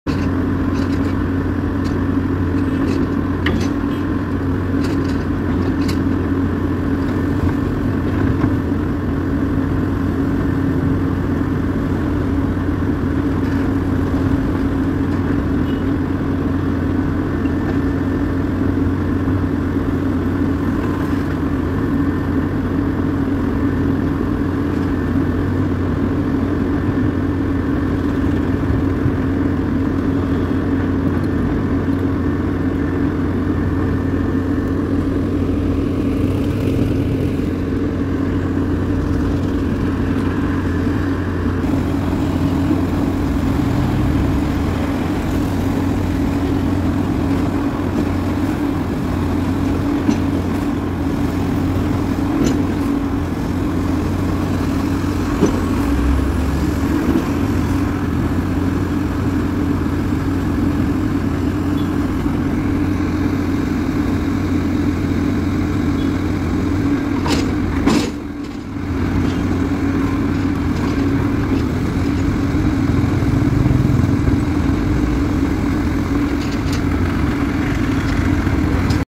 Earth drilling for high way expansion in kerala|Ernakulam
ദേശീയ പാത വികസനത്തിനായി ഫില്ലർ താഴ്ത്താൻ ഭൂമി തുറക്കുന്നു